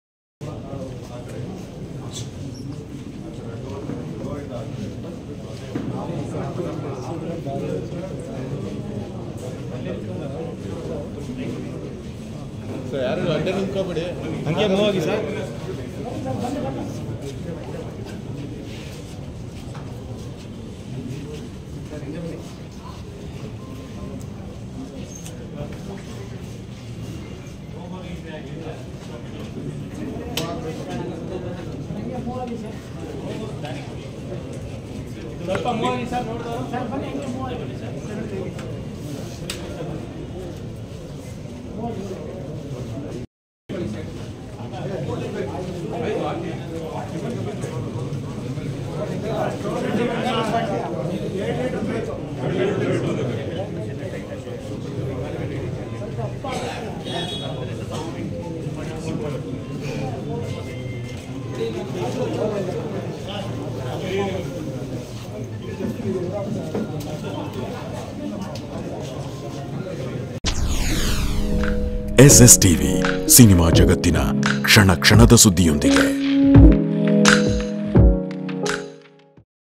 ಶಿವರಾಂ ಪಾರ್ಥಿವ ಶರೀರದ ಮುಂದೆ ಸಂಬಂಧಿಕರ ಆಕ್ರಂಧನ | K Shivaram | K Shivaram House | Family | SStv
bangalore
Karnataka fans
Production by:-
SStv
-: SStv PRODUCTIONS :-